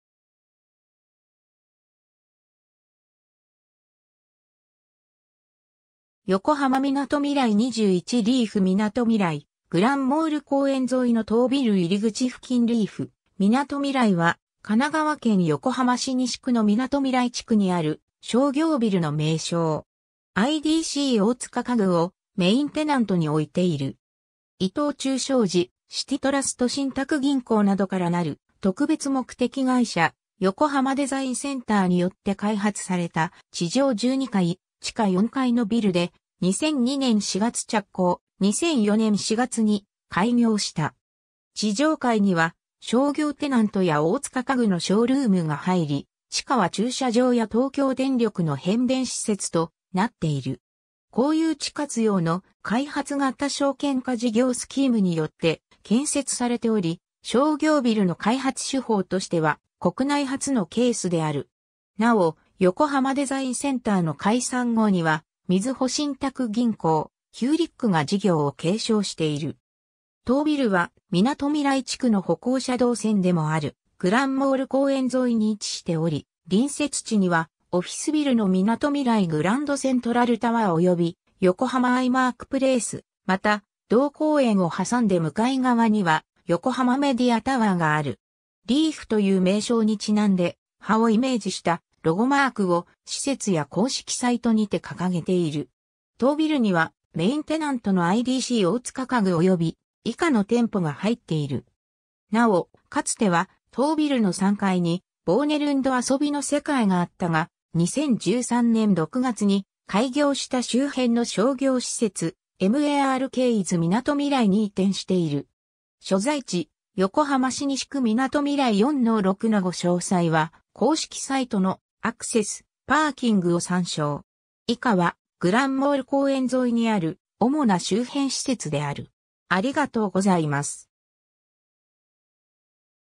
リーフみなとみらい
横浜みなとみらい21 」 リーフみなとみらい グランモール公園沿いの当ビル入り口付近（ガラス張りのアトリウム構造） リーフみなとみらい (Leaf minatomirai) は、神奈川県横浜市西区のみなとみらい地区（42街区の北区画・グランモール公園沿い）にある商業ビル（およびそのビル内における商業施設）の名称。
IDC大塚家具をメインテナントに置いている。
 伊藤忠商事、シティトラスト信託銀行（2006年営業廃止）などからなる特別目的会社「横浜デザインセンター」によって開発された地上12階（高さ約70m）・地下4階のビルで、2002年4月着工、2004年4月に開業した。
地上階には商業テナントや大塚家具のショールームが入り、地下は駐車場や東京電力の変電施設となっている。
公有地活用（賃借）の開発型証券化事業スキームによって建設されており、商業ビルの開発手法としては国内初のケースである。
なお、横浜デザインセンターの解散後にはみずほ信託銀行、ヒューリック（旧シルクポート）が事業を継承している。
当ビルはみなとみらい地区の歩行者動線でもあるグランモール公園沿い（西側）に位置しており、隣接地にはオフィスビルのみなとみらいグランドセントラルタワーおよび横浜アイマークプレイス、また同公園を挟んで向かい側には横浜メディアタワーがある（後節の「周辺施設」も参照）。
 なお、かつては当ビルの3階に「ボーネルンドあそびのせかい」（横浜みなとみらい店）があったが、2013年6月に開業した周辺の商業施設「MARK IS みなとみらい」に移転している。
 以下はグランモール公園沿いにある主な周辺施設である。